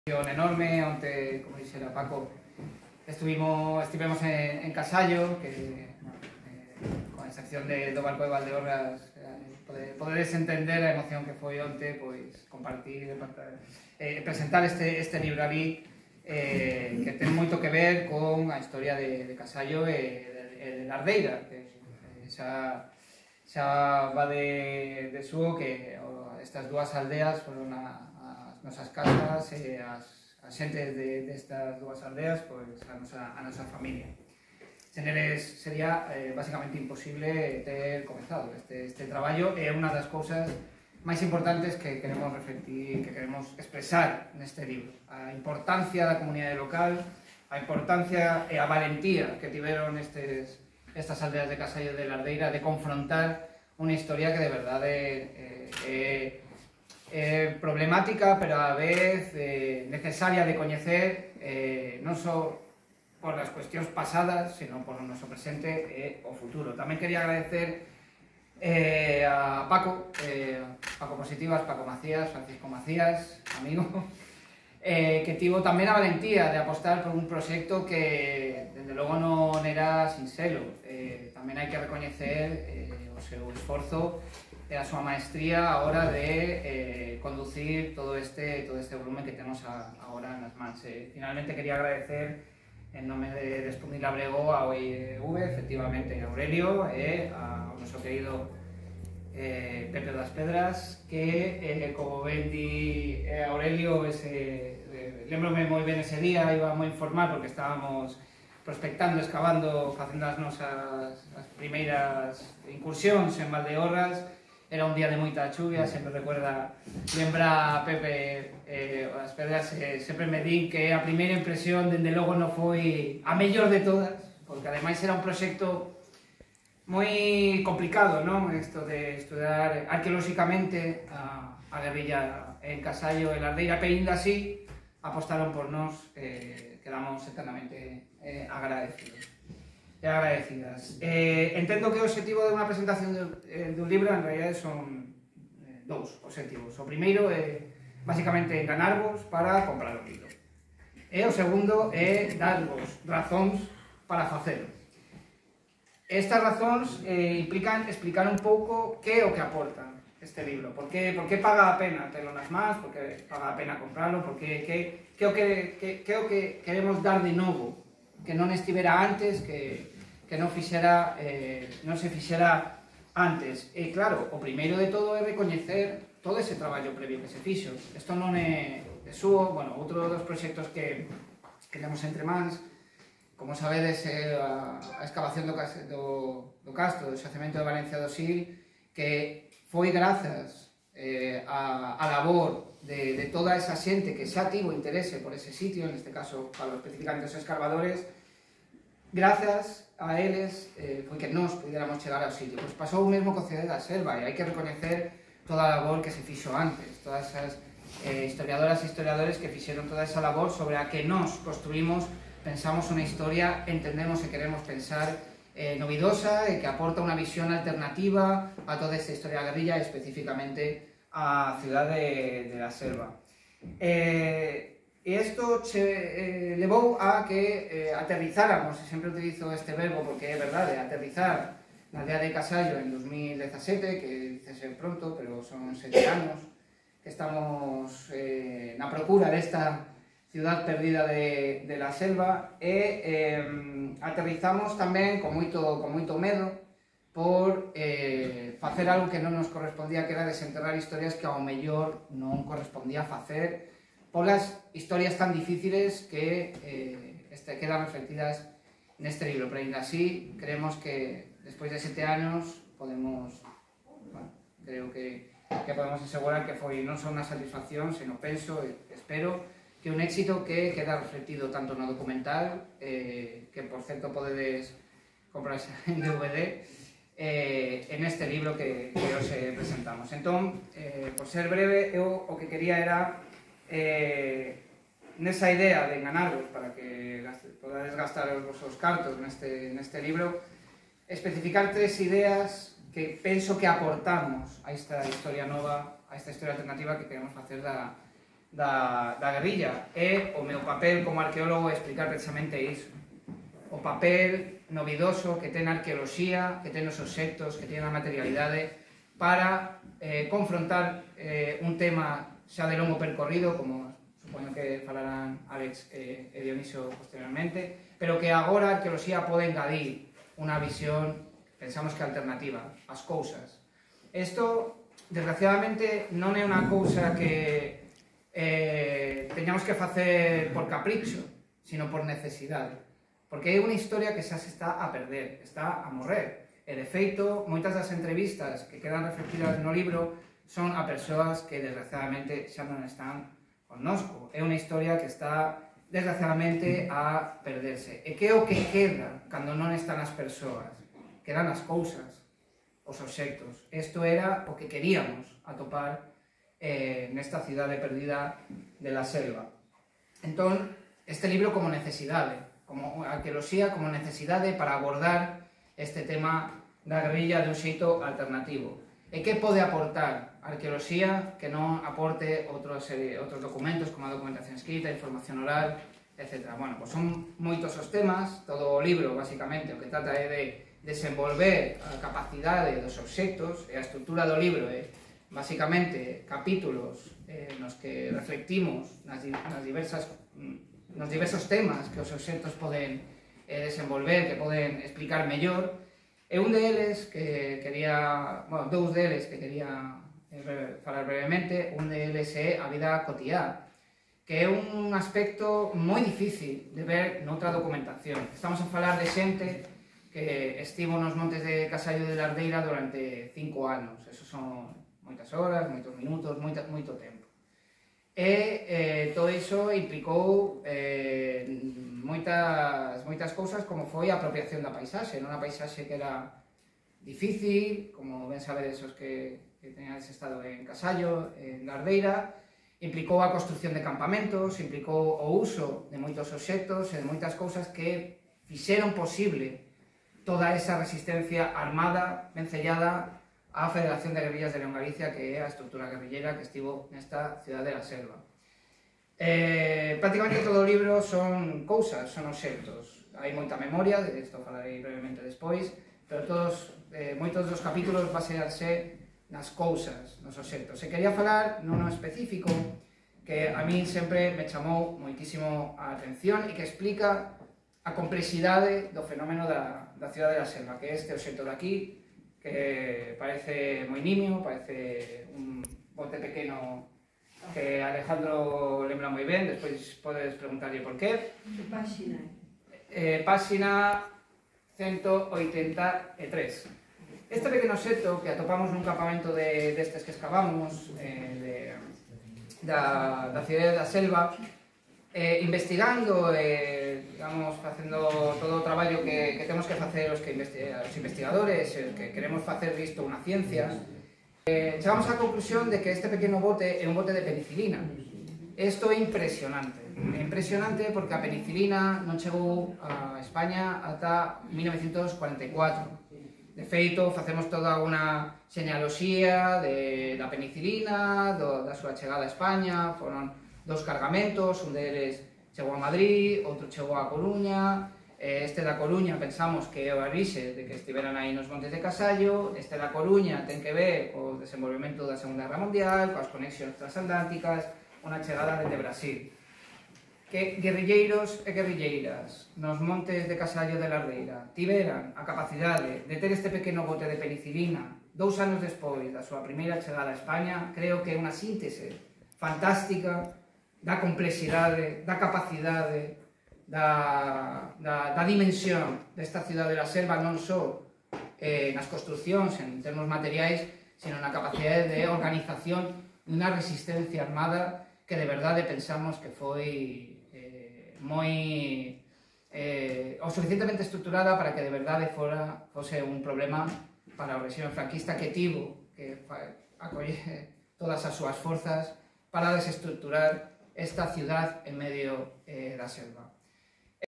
El arqueólogo Carlos Tejerizo explica las claves del libro "Ciudad de La Selva"
El arqueólogo Carlos Tejerizo, que Sputnik Labrego, desgranó las claves del libro "Ciudad de la Selva", que recoge la investigación de este grupo científico sobre la localización de campamentos usados por la guerrilla antifranquista en Casaio (Carballeda de Valdeorras) y que fue presentado en esta localidad y en O Barco de Valdeorras.